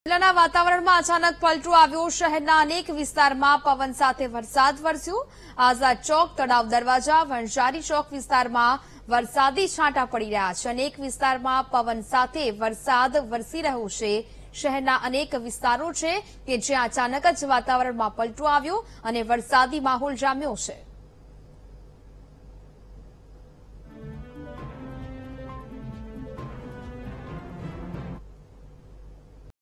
Junagadh News: વાતાવરણમાં પલટો, શહેરના અનેક વિસ્તારમાં પવન સાથે વરસાદ
આઝાદ ચોક,તળાવ દરવાજા,વણઝારી ચોક વિસ્તારમાં છાંટા, શહેરના અનેક ભાગમાં પવન સાથે વરસાદી માહોલ | VTV Gujarati